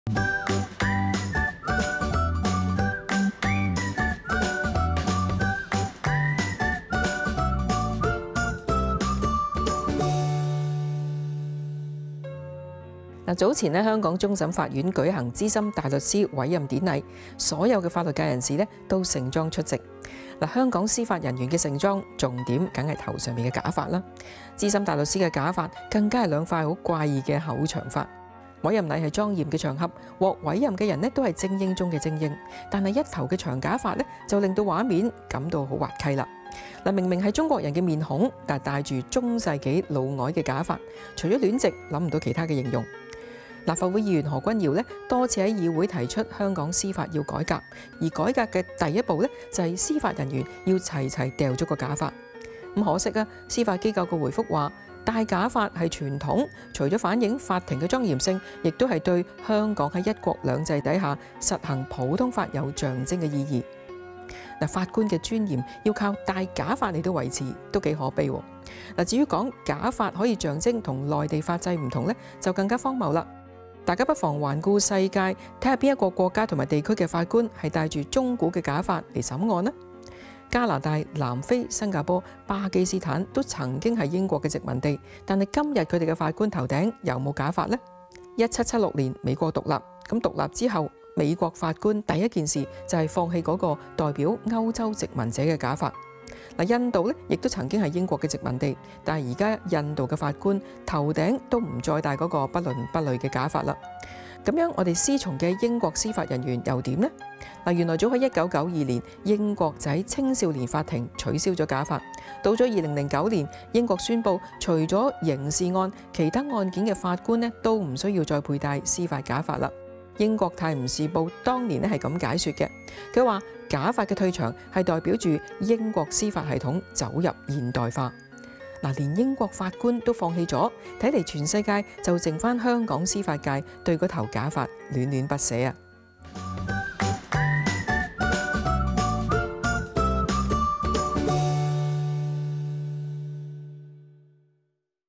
有理說得清｜頭頂上的滑稽｜屈穎妍
早前香港終審法院舉行資深大律師委任典禮，所有法律界人士都盛裝出席。香港司法人員的盛裝，重點是頭上假髮，資深大律師的假髮更是兩塊怪異的厚長髮。委任禮是莊嚴場合，獲委任者是精英中的精英，然而，一頭長假髮，卻令畫面徒添滑稽感。明明是中國人臉孔，卻戴著中世紀老外假髮，除了戀殖，想不到其他形容。立法會議員何君堯多次在議會提出香港司法要改革，而改革的第一步，就是司法人員齊齊甩掉假髮。可惜，司法機構回覆：戴假髮是傳統，除反映法庭莊嚴性，也對香港在一國兩制下實行普通法有象徵意義。法官的尊嚴要靠戴假髮來維持，實在可悲。至於說假髮可象徵與內地法制不同，就更荒謬了。大家不妨環顧世界，看看哪些國家及地區的法官是戴著中古假髮來審案？加拿大，南非，新加坡，巴基斯坦曾是英國殖民地，但今天他們的法官頭頂有假髮嗎？1776年美國獨立，獨立後美國法官第一件事就是放棄那代表歐洲殖民者的假髮。印度曾是英國殖民地，但如今印度法官的腦袋都沒再戴那不倫不類的假髮。那麼，我們師從的英國司法人員又如何呢？原來早在1992年英國就在青少年法庭取消假髮；到了2009年，英國宣佈除了刑事案，其他案件法官均不需要佩戴司法假髮。英國《泰晤士報》當年是這樣解說：「假髮的退場，代表英國司法系統走進現代化。」連英國法官都放棄了，看來全世界就只剩香港司法界對那頭假髮戀戀不捨。